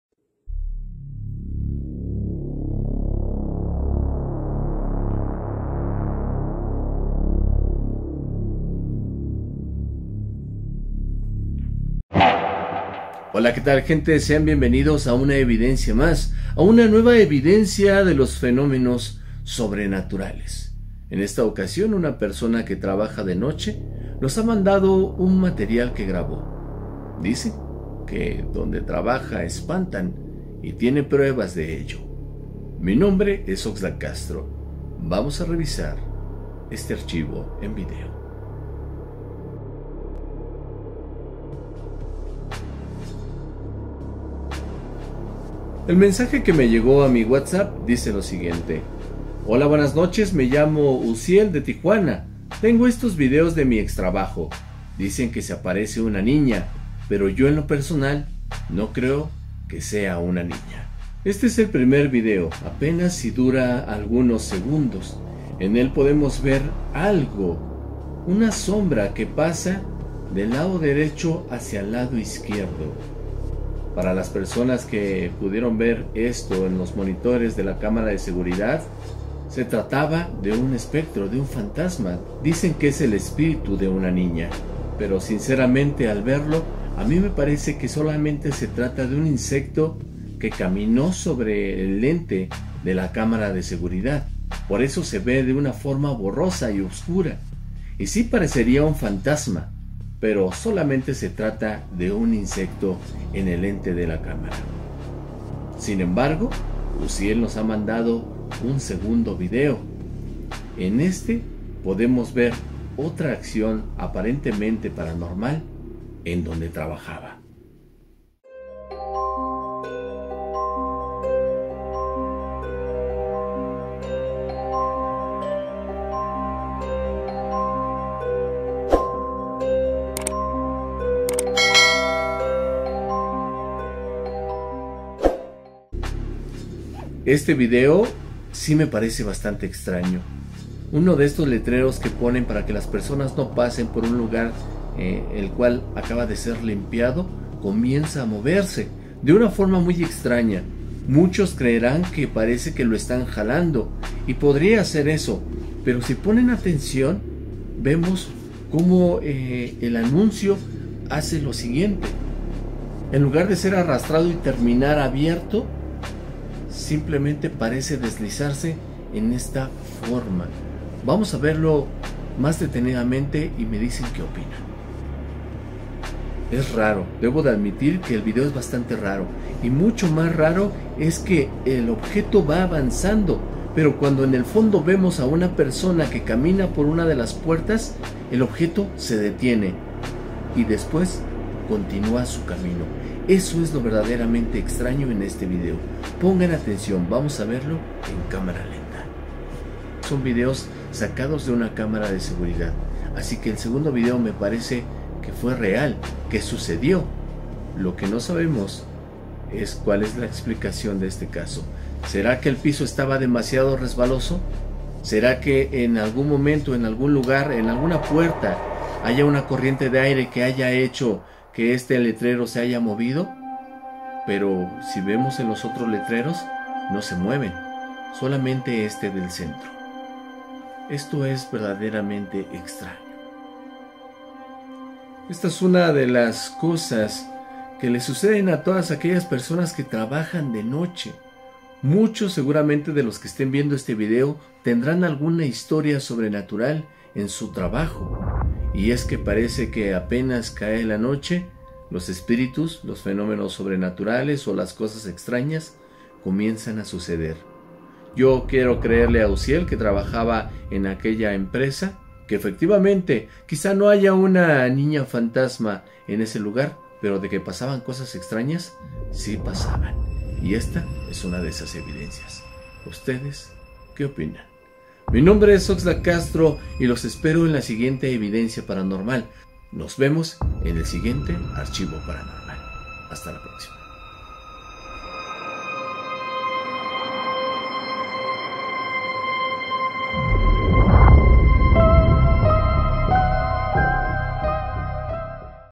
NIÑA FANTASMA 🧟‍♀️ ESPANTA en este TRABAJO 😲
En este video Oxlack Investigador te dará a conocer el caso extraño o paranormal de una niña fantasma que hace cosas paranormales en un trabajo

MI TIK TOK: @oxlackinvestigador
MI TWITTER: @OxlackCastro

PUEDES CONTACTARME EN EL CORREO